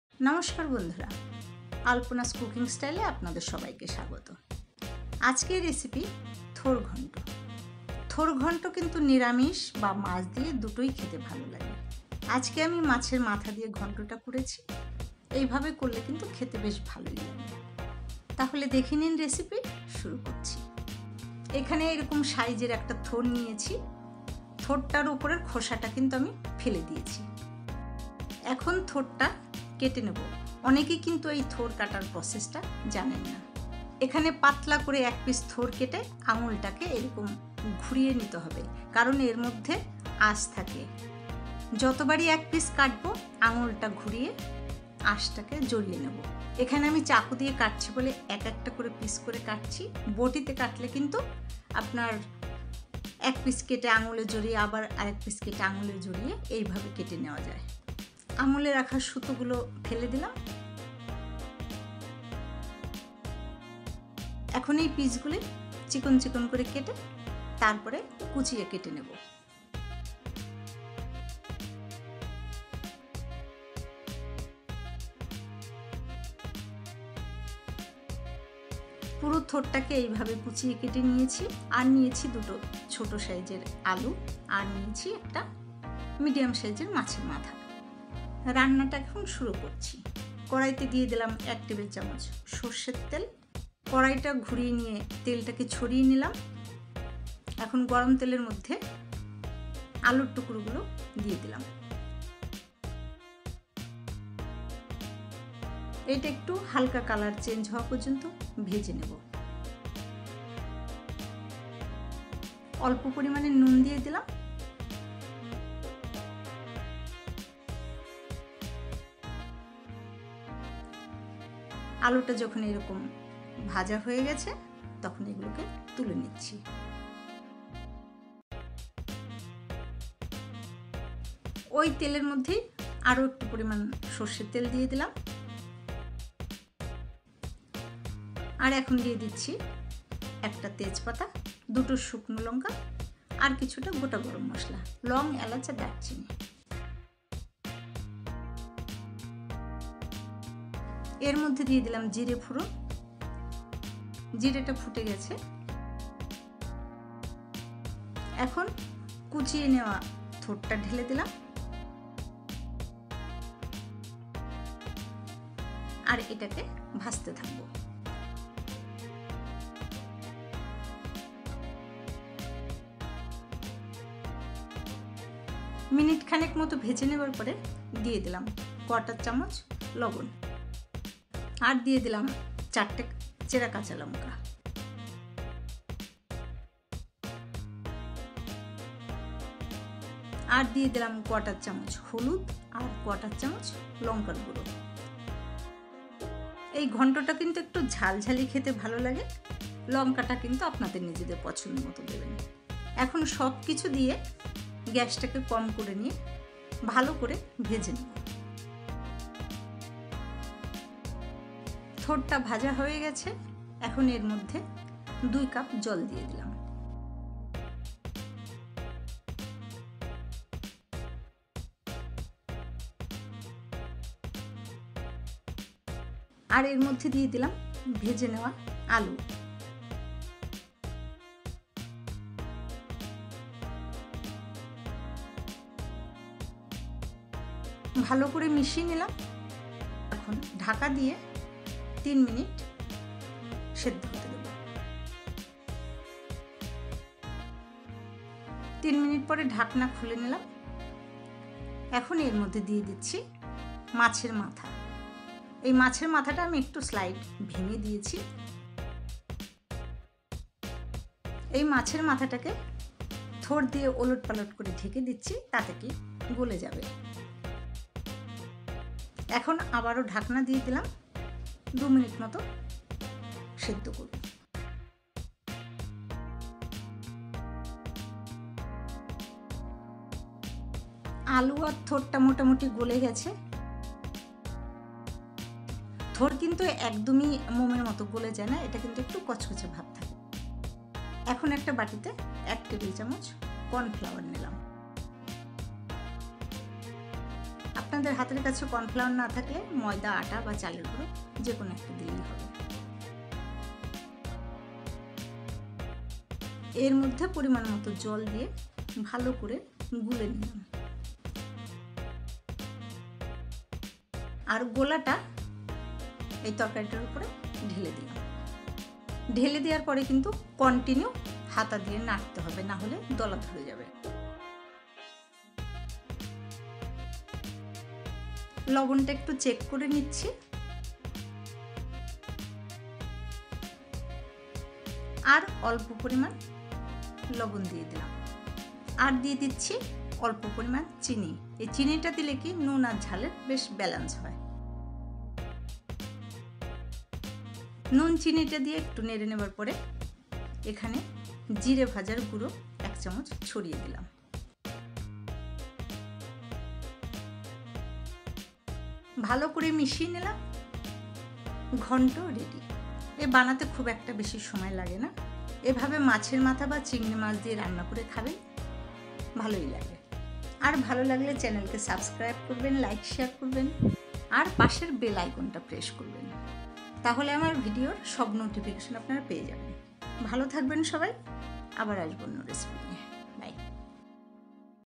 Thor Ghonto Macher Matha Diye | Banana Stem Recipe with Fish's Head in Bengali
Thor Ghonto is a special Bengali dish that is famous in West Bengal and Bangladesh. This recipe can be prepared in a vegetarian and non-vegetarian way. It has a unique taste that can't be found anywhere else.

Ingredients of Thor Ghonto Macher Matha Diye:

Banana stem - 7 inch
Potato - 2 small size
Fish's Head - Medium size
Fried Jeera Powder - 1 tsp
Mustard Oil as required
For Tempering, take 1 bay leaf, 2 dried red chilies, some whole garam masala, and Jeera